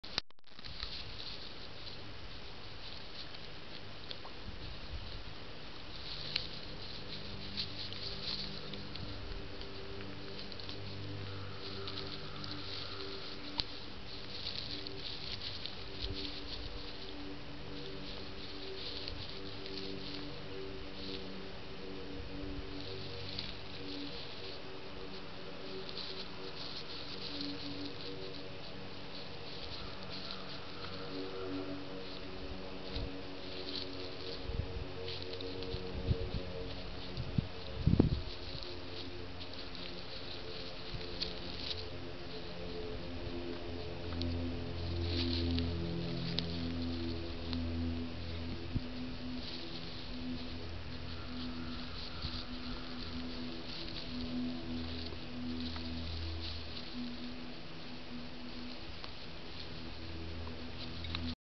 Nine-banded Armadillo rooting around at Halpata Tastanaki Preserve
Nine-banded Armadillo rooting around at Halpata Tastanaki Preserve near Withlacoochee River, shot with a Canon SX100